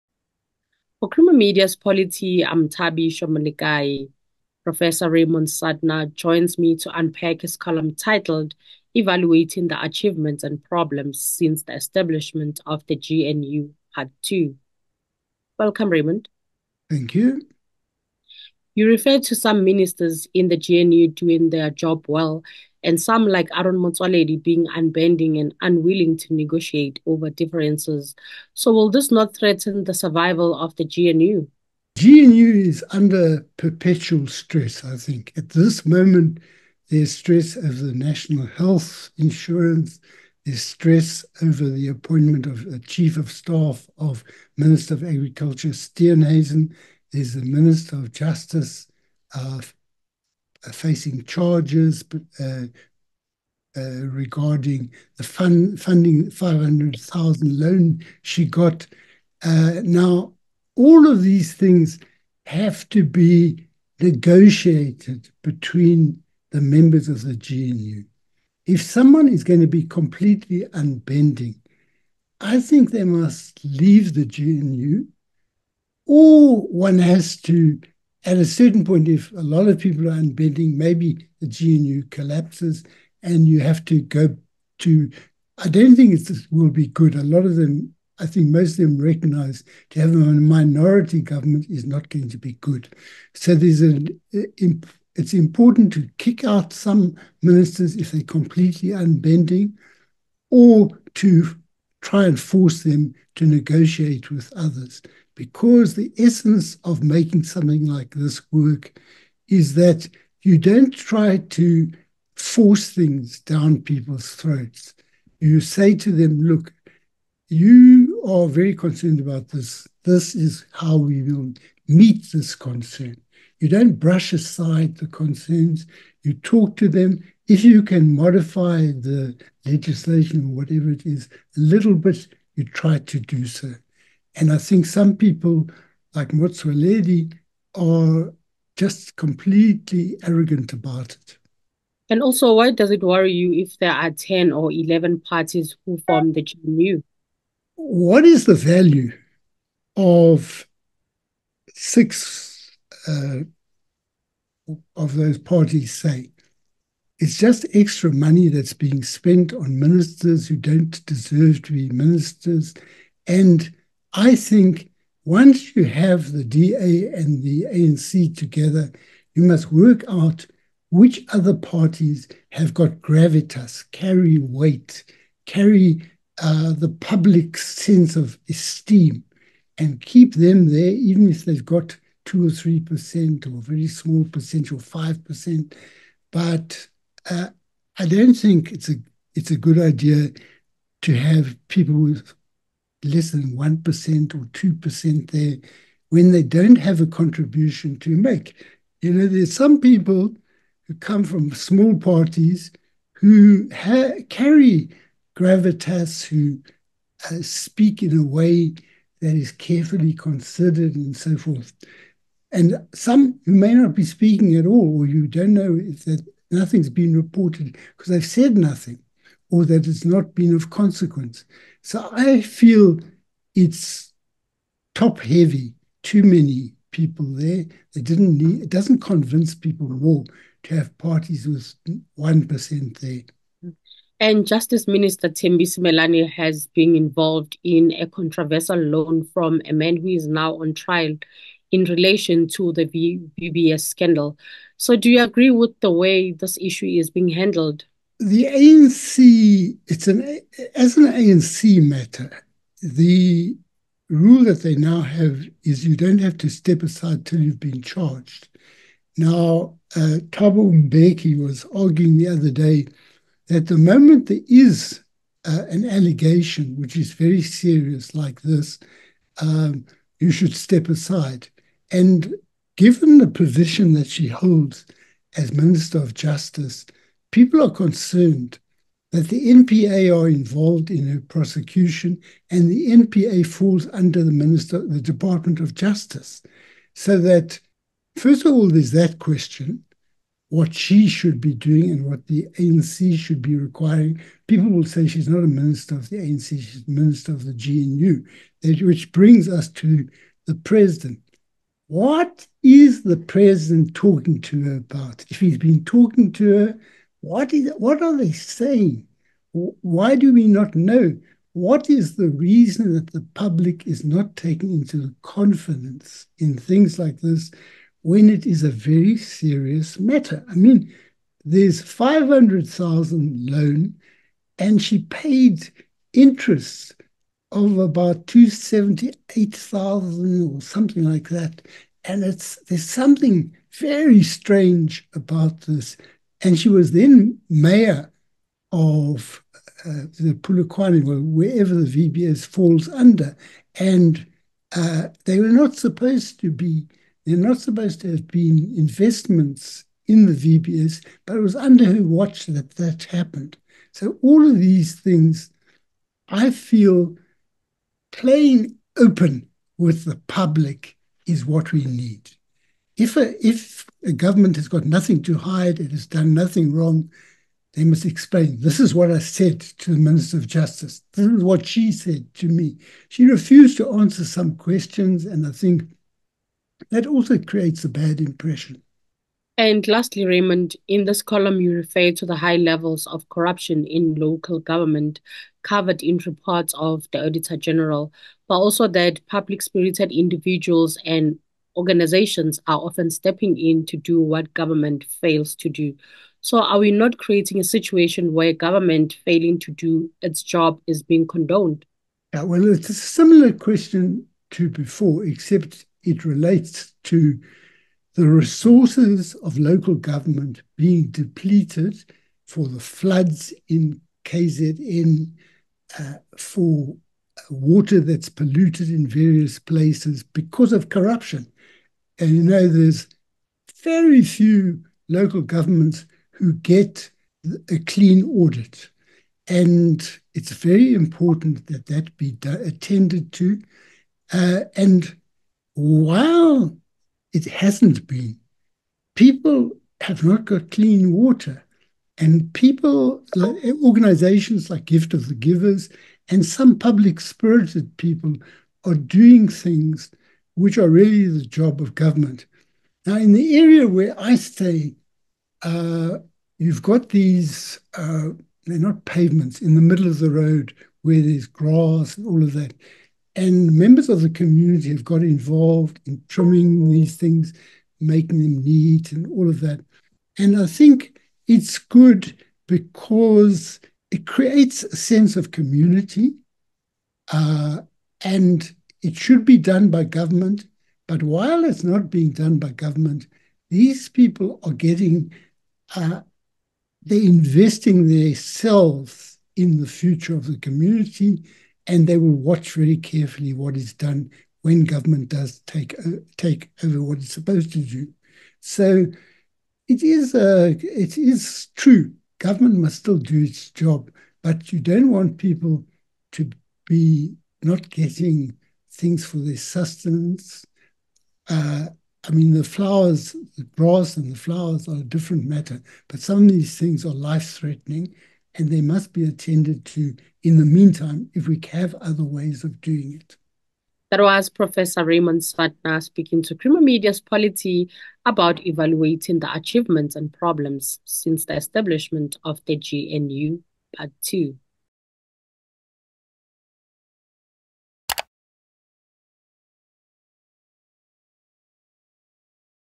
Evaluating the achievements and problems since the establishment of the GNU – Part Two
ANC/DA agreement made GNU possible. The reason why it was possible to create the Government of National Unity (GNU) and why it has an element of stability is that the two major minority parties, the ANC (receiving 40% of the vote) and the DA (just under 21%) decided to work together in a GNU.

Professor Raymond Suttner unpacks part two of his column, 'Evaluating the achievements and problems since the establishment of the GNU'.